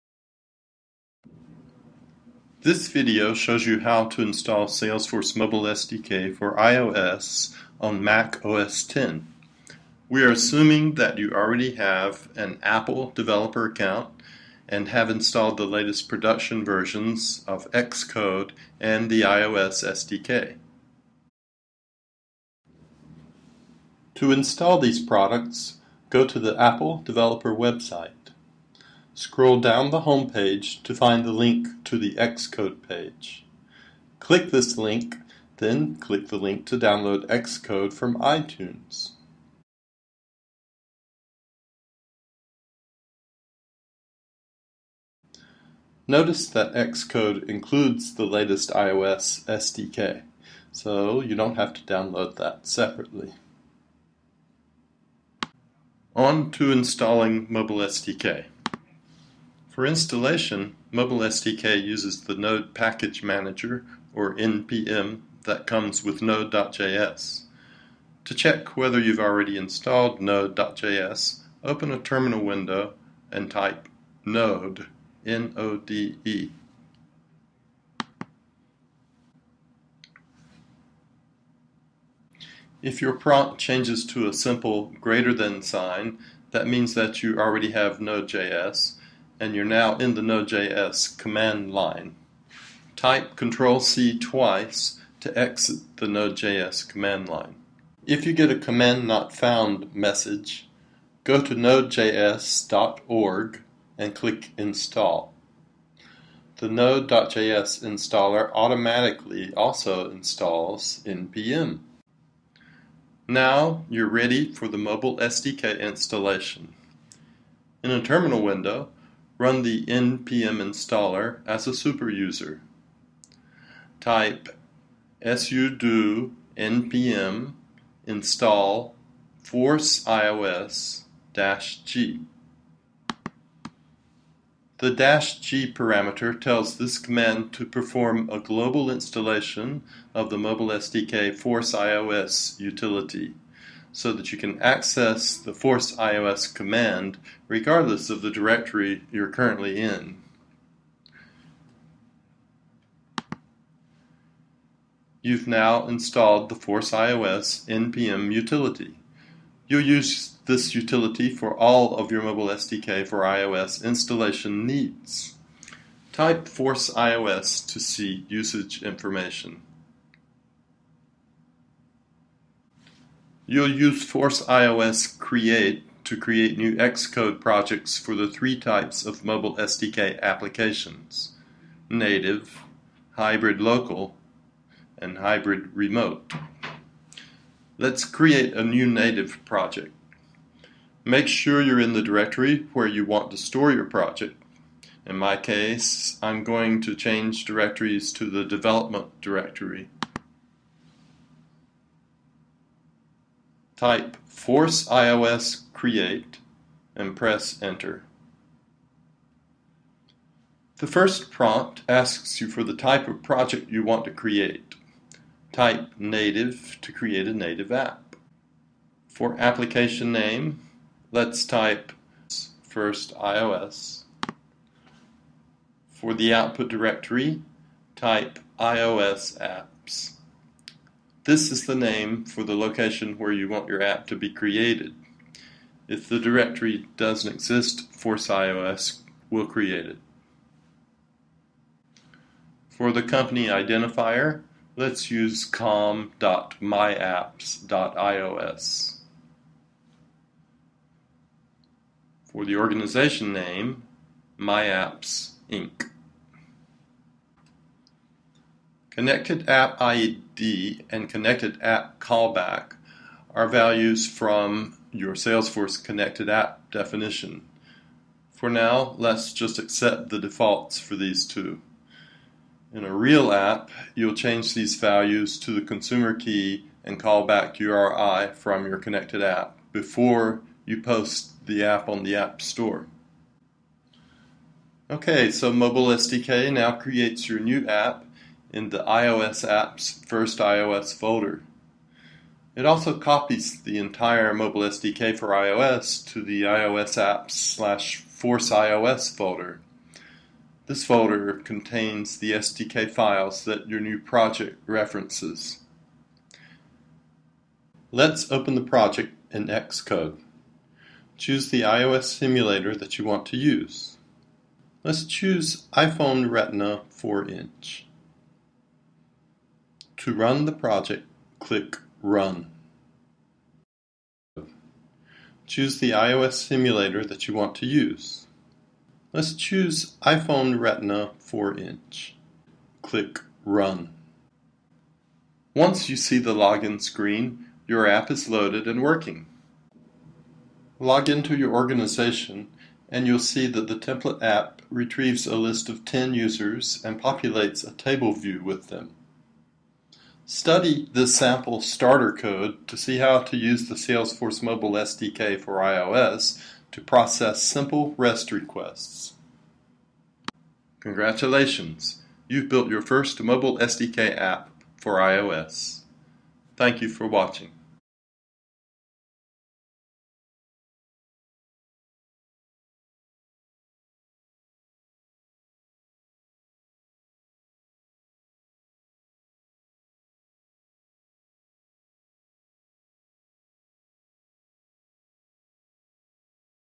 Installing Salesforce Mobile SDK for iOS on Mac OS X
Build your first native iOS app with Salesforce Mobile SDK. This video will help you setup all the developer tools (XCode, NPM and Mobile SDK) on an OS X environment. You would then create your first mobile app for iOS that connects to Salesforce.

Key Takeaways:
:: Install all the developer tools (XCode and NPM)
:: Install Salesforce Mobile SDK via NPM
:: Create you first native iOS mobile app